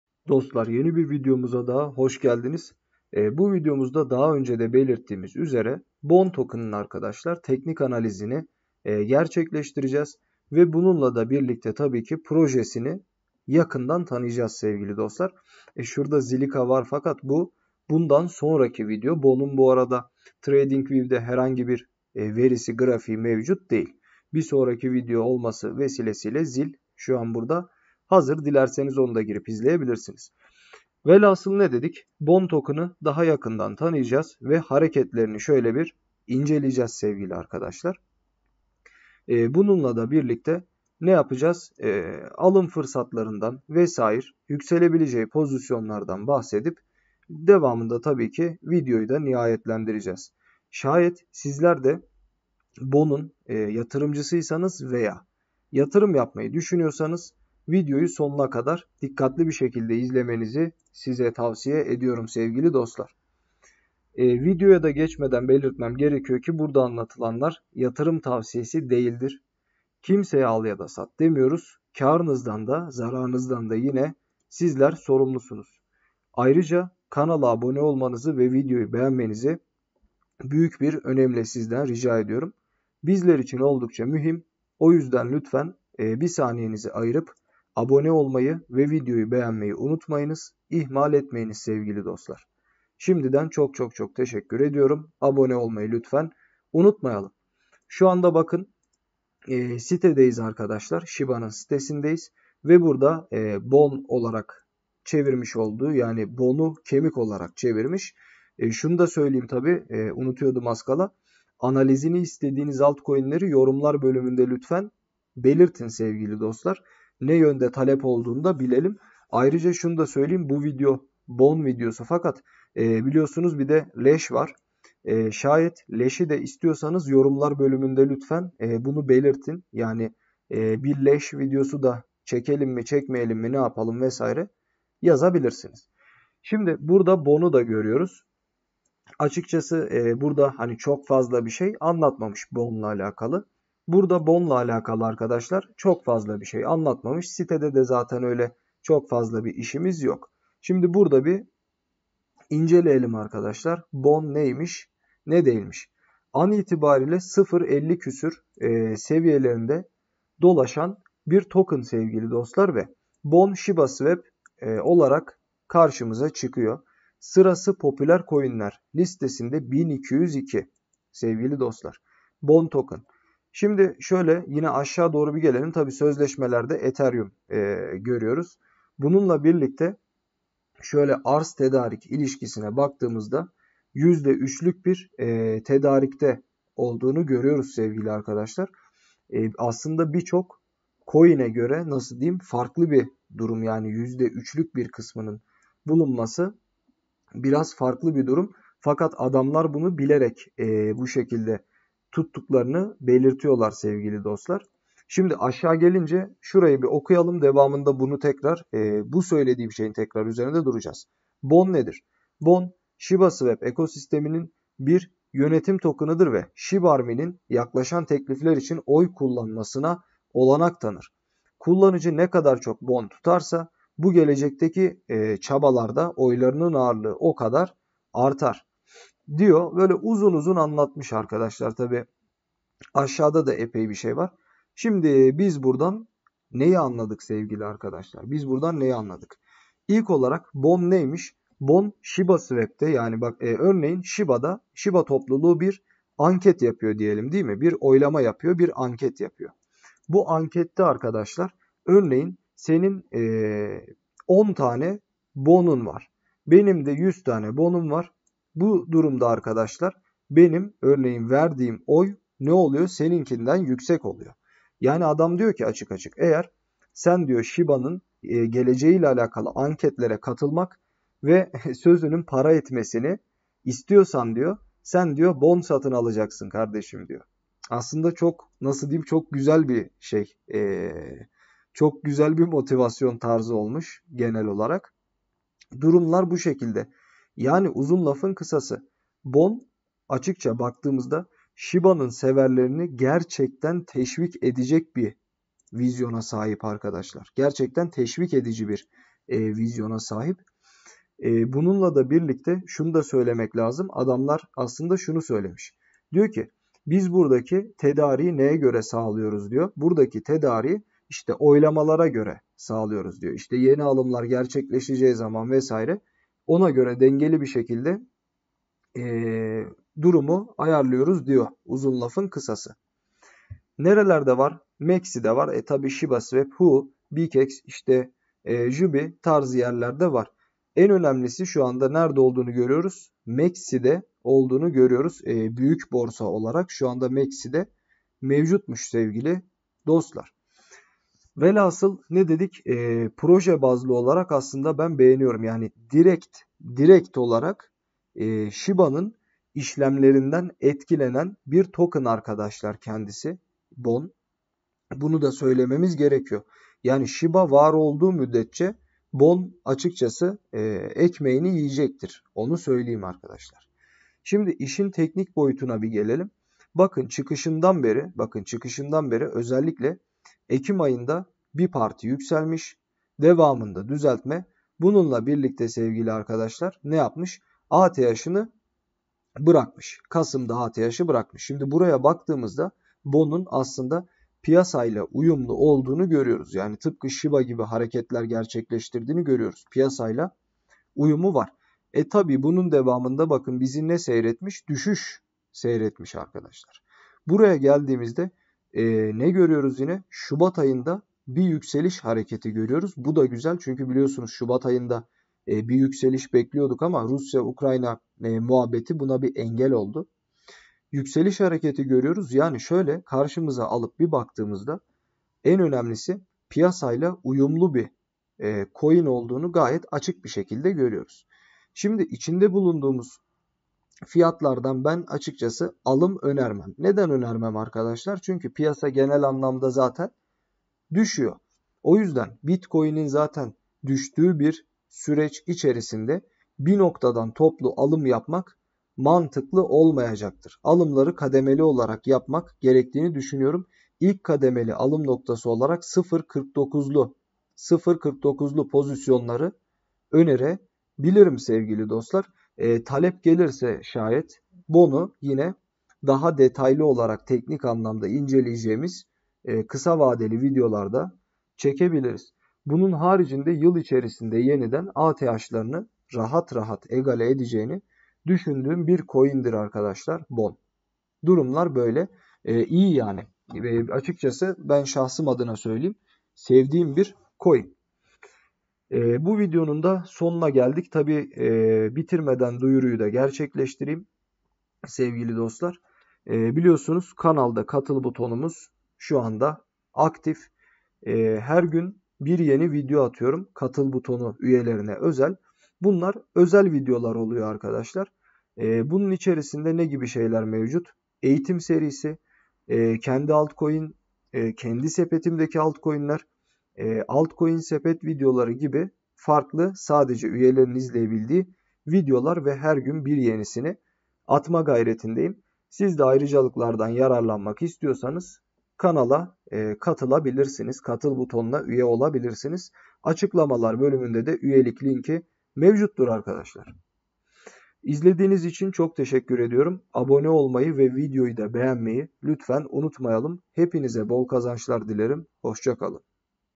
BONE COİN YÜKSELİŞ VADEDİYOR! TEKNİK ANALİZ!🚀BİR SHIBA TOKENI!🚀2022 YENİ ATH SEVİYELERİ🚀SON ŞANSLAR🚀
Bu videomuzda kripto borsalarında oldukça popüler bir coin olan Bone coin'i inceledik. Bone token'in destek ve direnç noktalarını belirterek kısa, orta ve uzun vade hedeflerinden söz ettik. Bone token alım ve satım noktalarını belirleyerek, hangi noktadan almalıyız? Gibi sorulara yanıt olmaya çalıştık. Bone, uzun vadede yakaladığı popülerite ve beklenen hacimle 2022'de çok büyük yükselişler yapabilir. Bone, bir shiba tokenıdır ve kitleye oy hakkı tanıma vizyonunu sahiplenmiştir. Bu videoda yatırım tavsiyesi verilmemiştir.